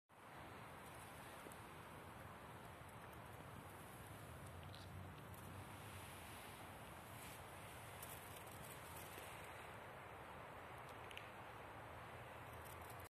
Fall Nature Squirrel LXIV
Hello, everyone!
I present to you this short video about a squirrel. Relax and cheer up!